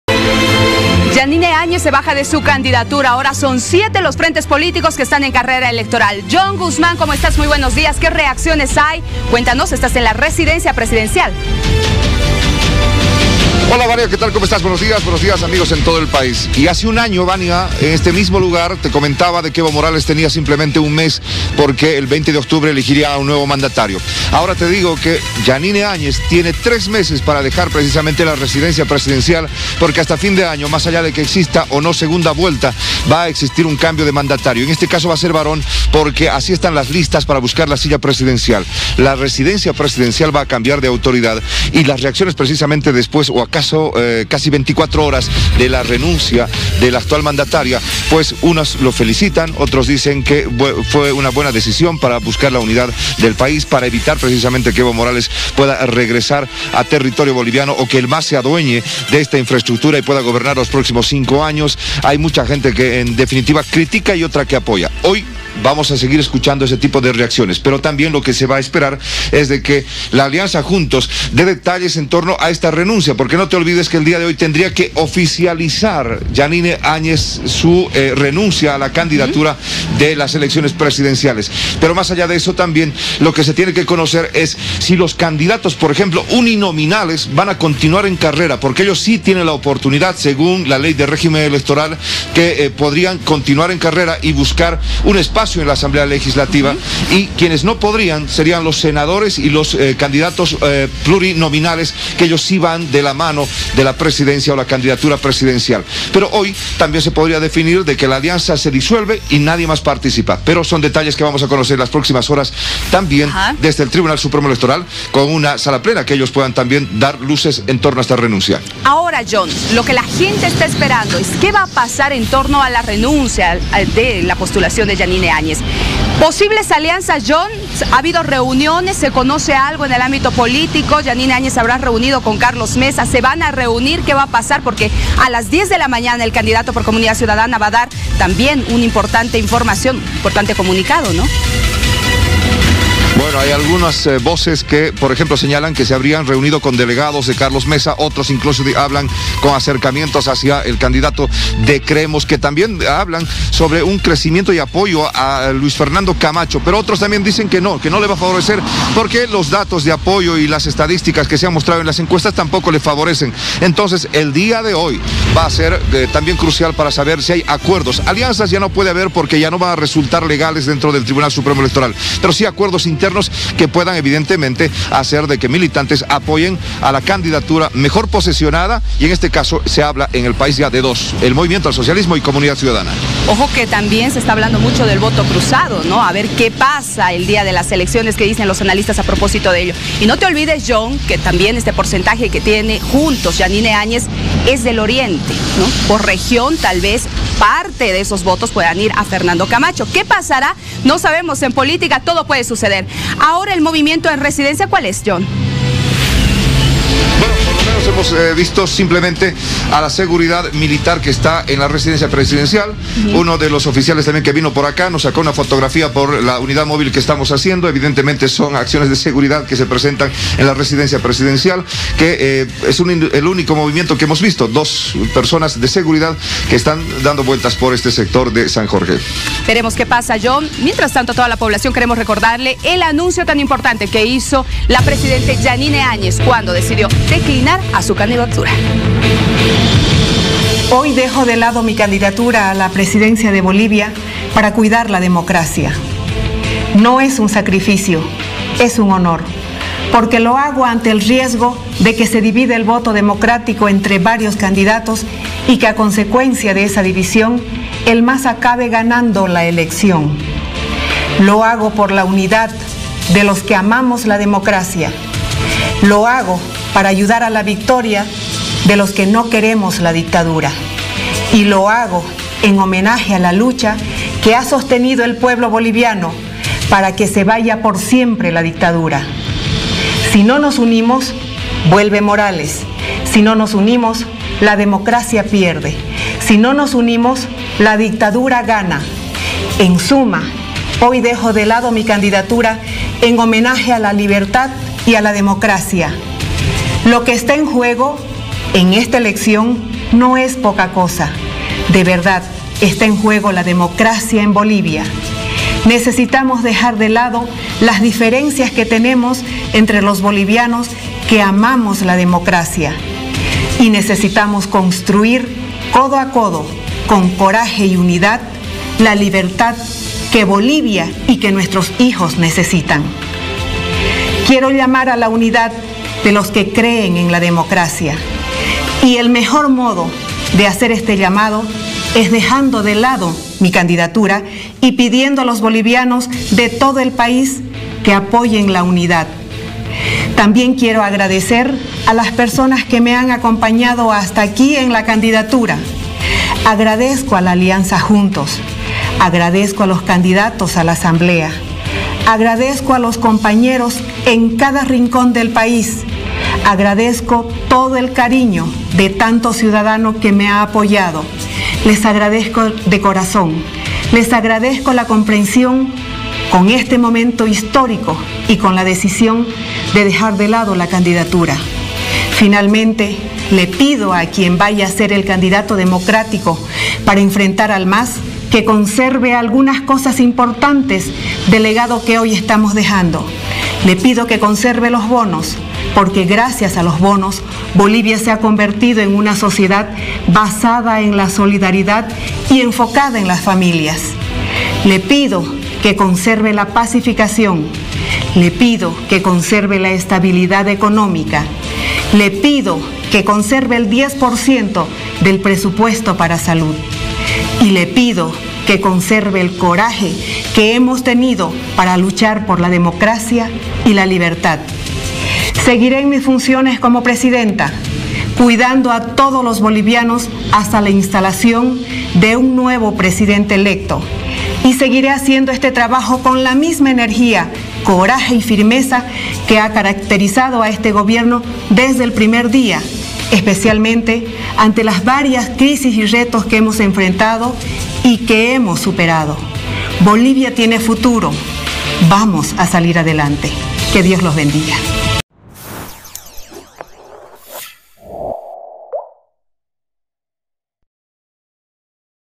Jeanine Áñez se baja de su candidatura presidencial
En un completo informe de ''Al Día'' le mostramos en detalle cómo fue el anuncio realizado por la Presidenta, Jeanine Áñez a su candidatura presidencial.